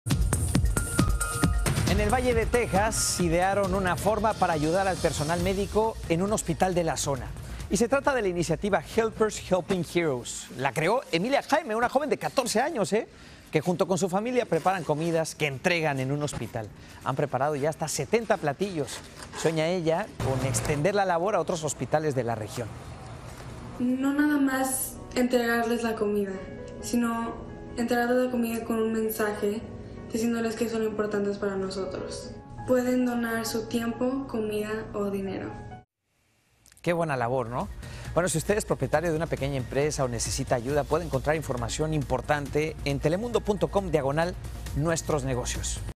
‘Helpers helping heroes’: iniciativa de una joven de 14 años para ayudar al personal de un hospital
Video oficial de Noticias Telemundo. El proyecto ideado por Emilia Jaime tiene como fin llevar comidas con mensaje a los trabajadores de un hospital de su localidad. La joven sueña con extender esta propuesta a otros centros médicos.